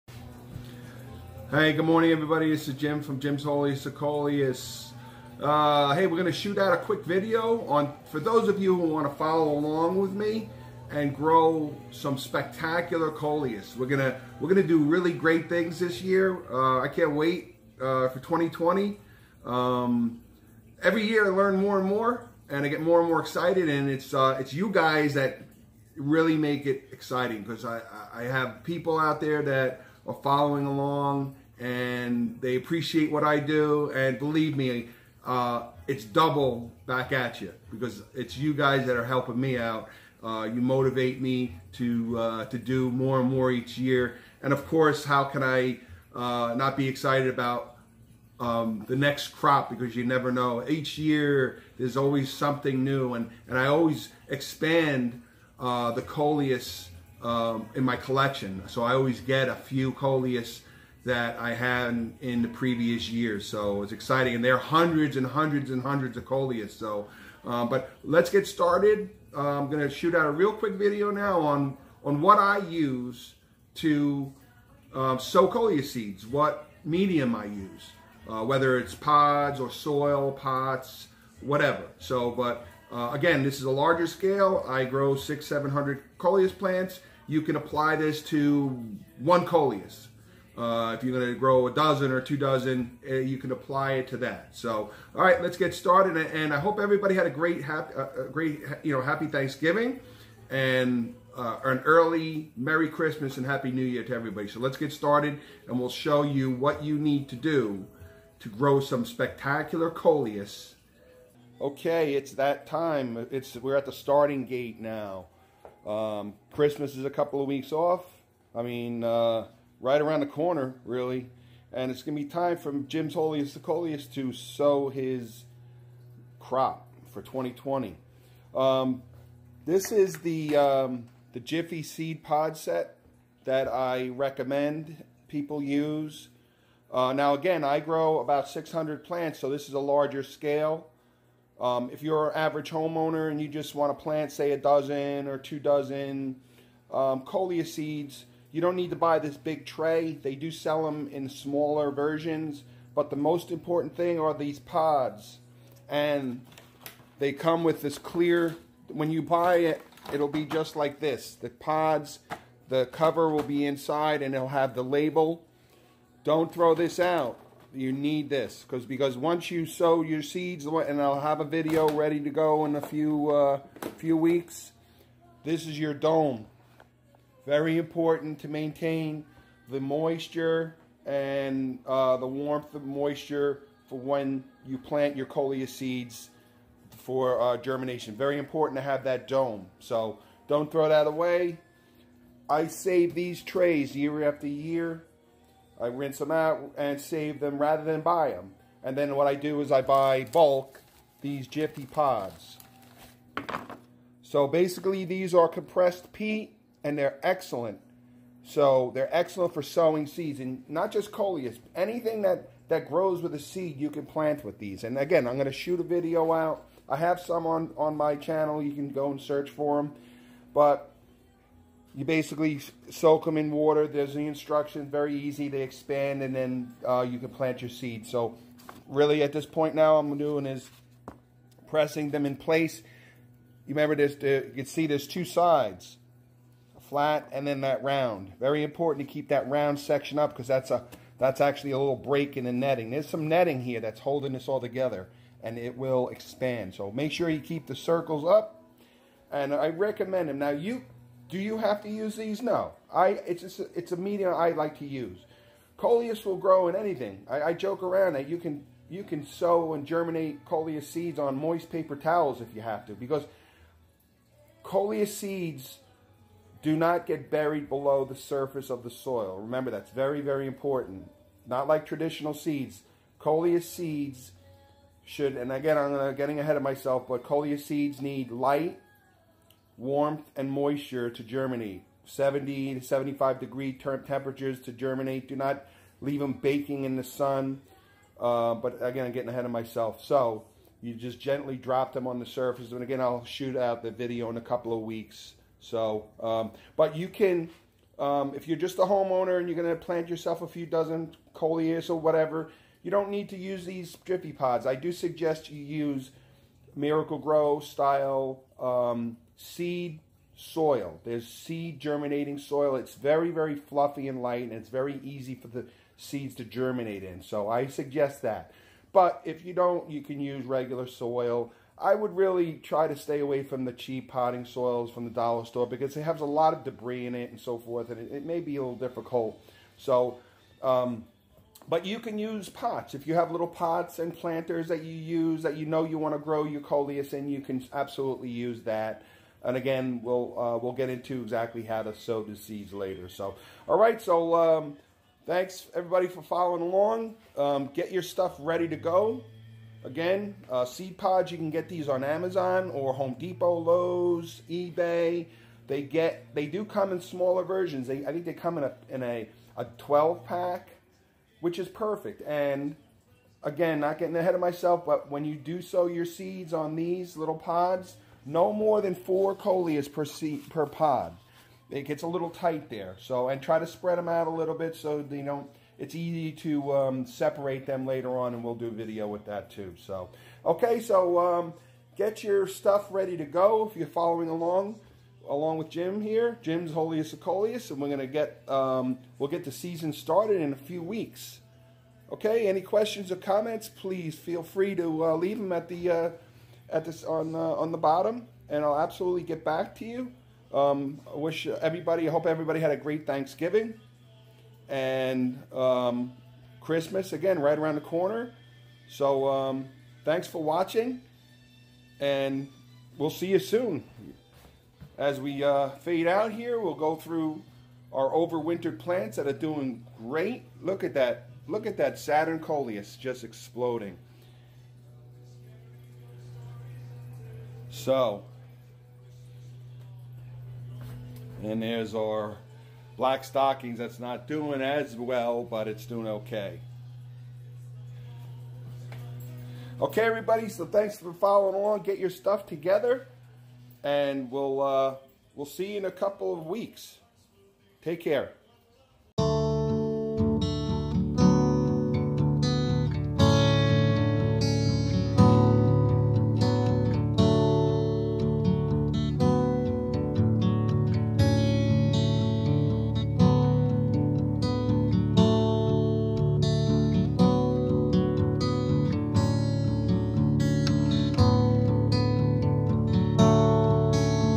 Jiffy Pod Tray system for Coleus Seeding.
Kicking Off my 2020 crop is right around the corner as Jim's Wholiest of Coleus prepares for Christmas day. The day I plant my 600 Coleus seeds. Here I show and suggest different methods to use to sow Coleus seeds.